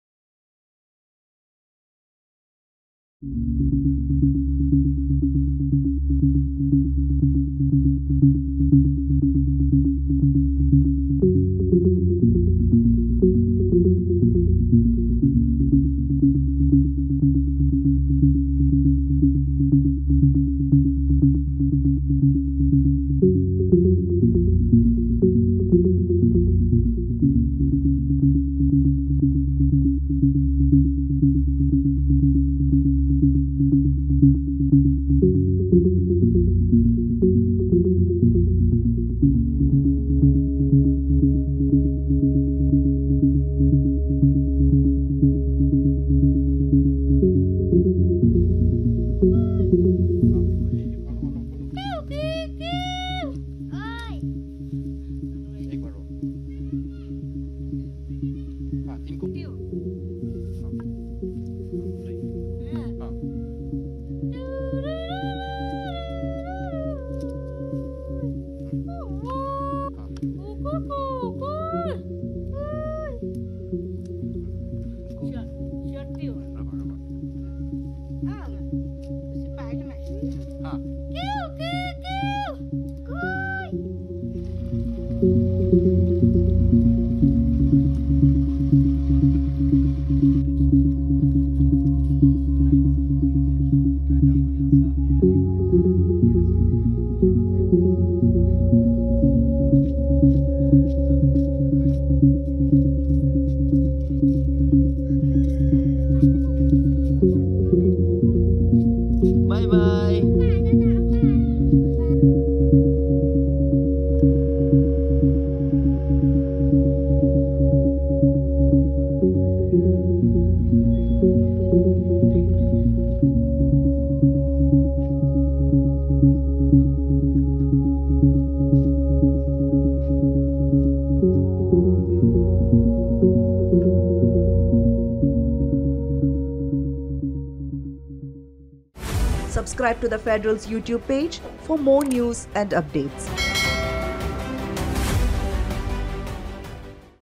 The Whistling Village: Locals use whistling to communicate | Meghalaya | Kongthong | The Federal
In Meghalaya's Kongthong village, people do not call each other by their names but communicate with a distinct melody or special tune. The people usually whistle or use some sort of singing to send out a message to fellow villagers. So, the area is known as 'Whistling Village'.